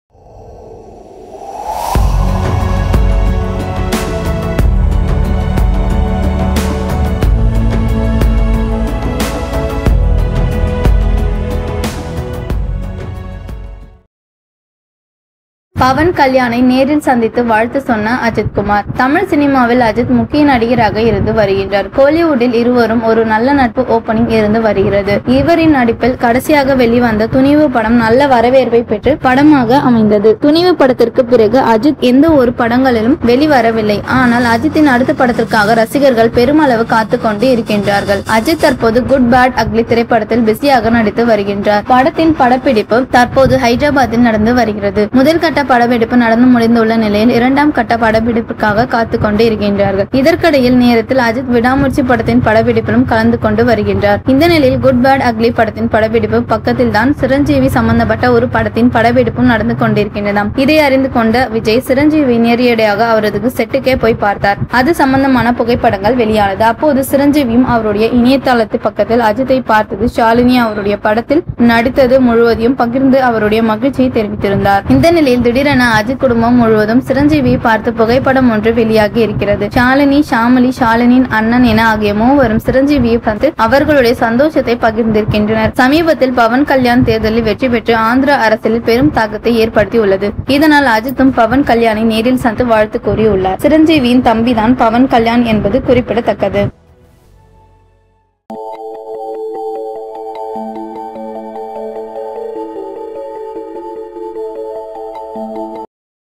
பவண் கல்யானை நேரில் சந்தித்து வாழ்த்து சொன்ன அஜித் | Ajith Family Meet Chiranjeevi & Pawan Kalyan
பவண் கல்யானை நேரில் சந்தித்து வாழ்த்து சொன்ன அஜித் | Ajith Family Meet Chiranjeevi & Pawan Kalyan | Shalini Ajith